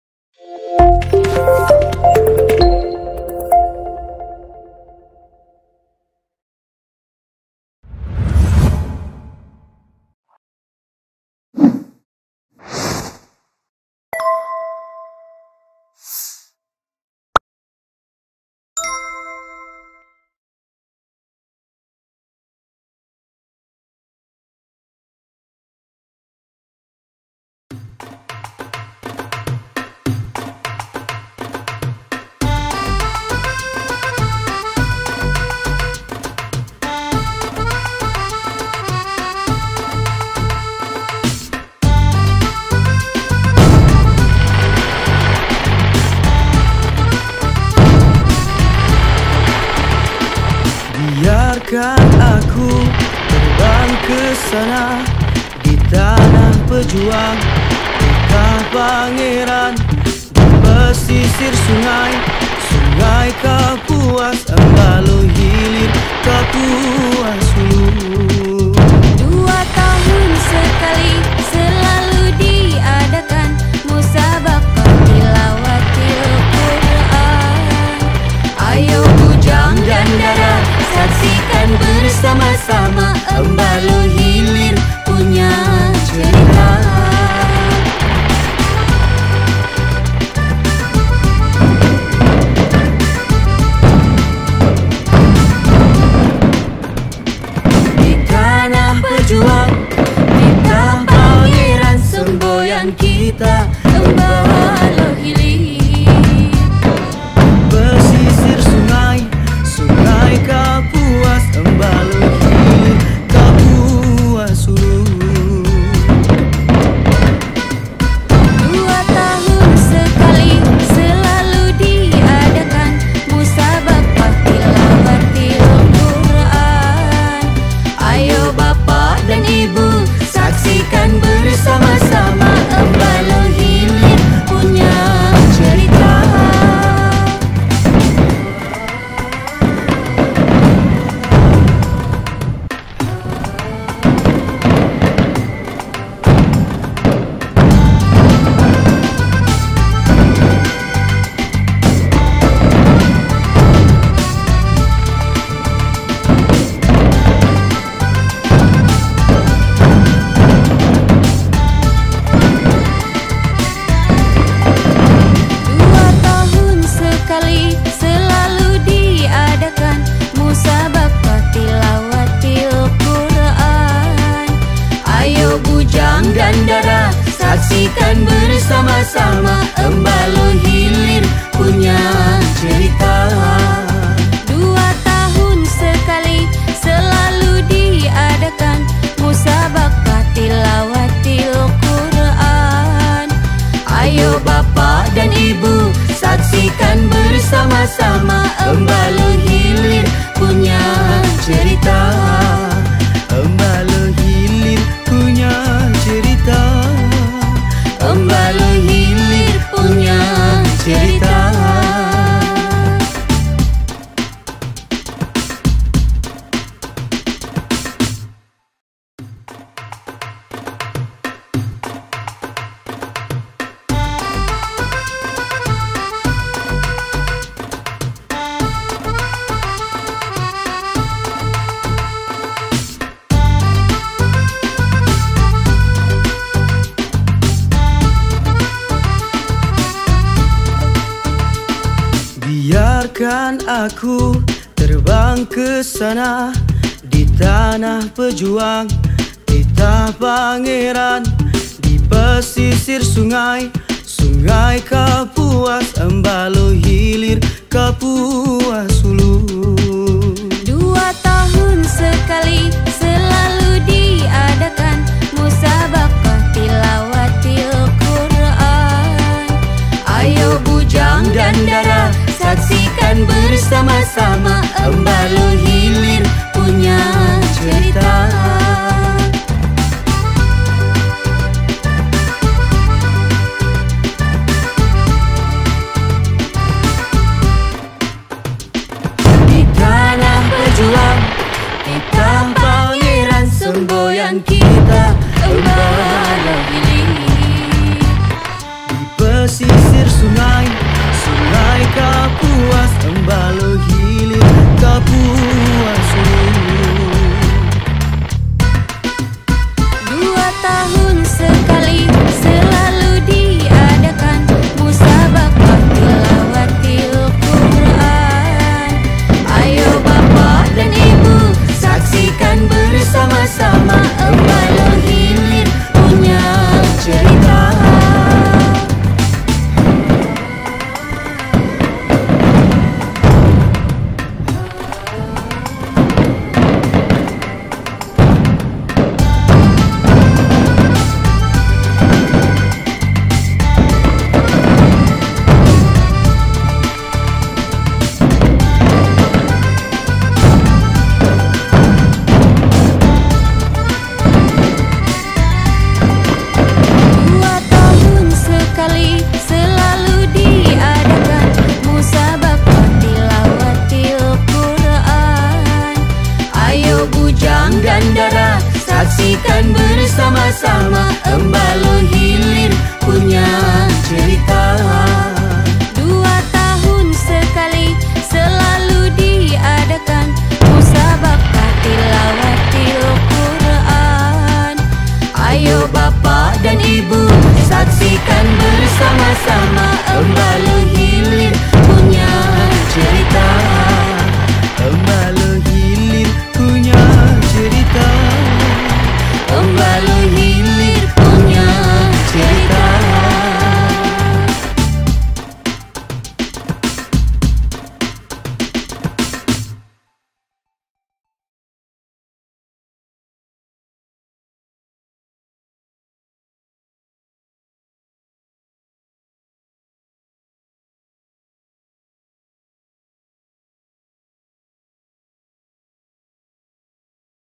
Best Fireworks "Closing Ceremony " MTQ 29 Kapuas Hulu - Embaloh Hilir Tahun 2022 | Versi Drone
Best Fireworks "Closing Cremony " MTQ 29 Kapuas Hulu - Embaloh Hilir Tahun 2022 | Versi Drone
inilah kemeriahan kembang api penutupan MTQ ke 29 tingkat kabupaten Kapuas Hulu di Kecamatan Emaloh Hilir Tahun 2022. Dalam kali ini kami sajikan versi drone.